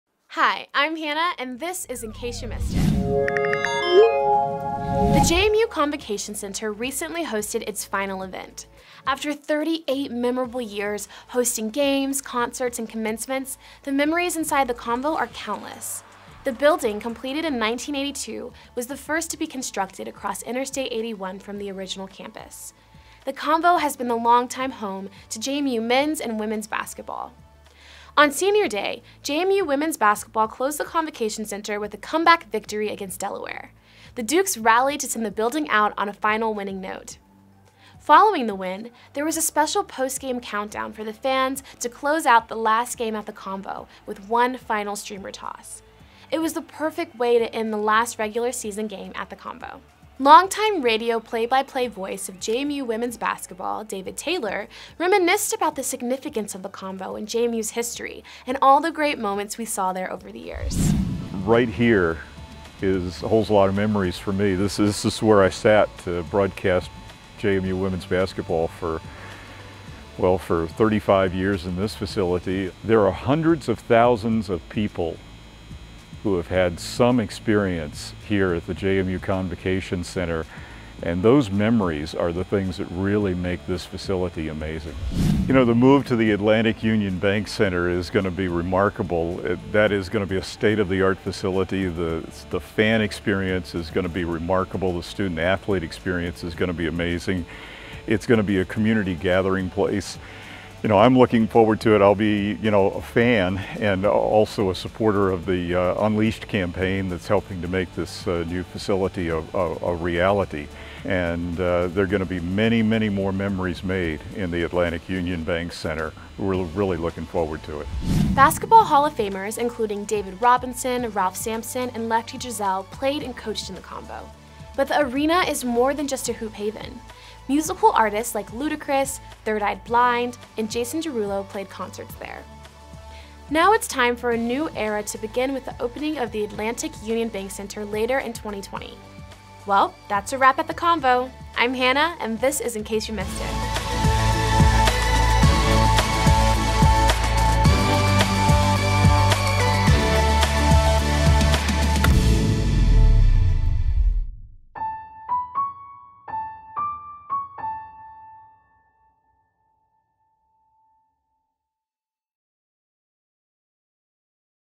ICYMI: JMU Convocation Center Closes Its Doors
The third installment of "ICYMI" highlights the JMU Convocation Center, which hosted events for 38 years. Join us in saying farewell to the Convocation, as JMU preps for the new Atlantic Union Bank Center.